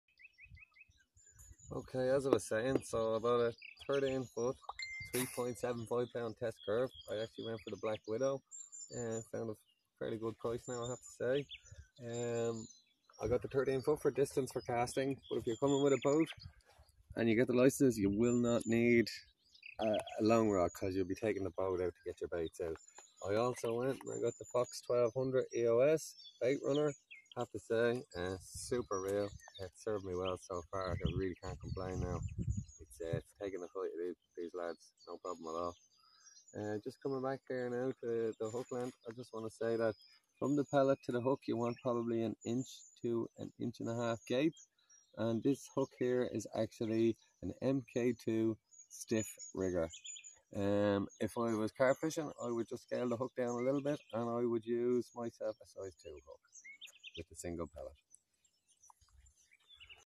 The Catfish setup we use P.3
Our rig setup for targeting Catfish part 3.